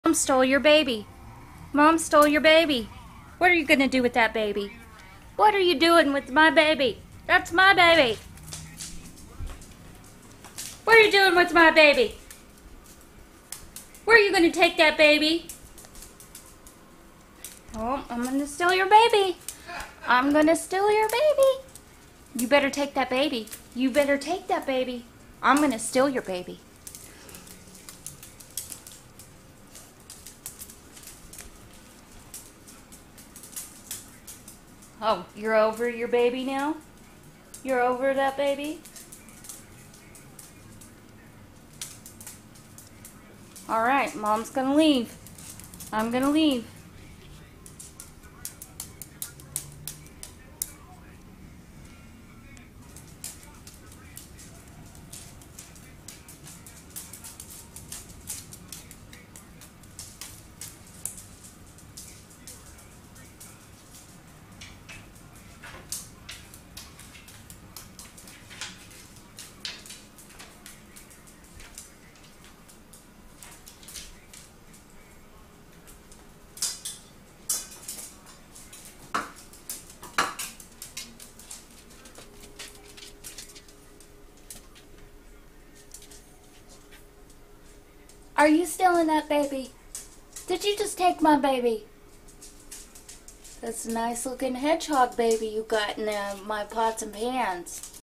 Hilarious Hex the skunk steals a hedgehog baby
Hilarious Hex steals Hedgehog baby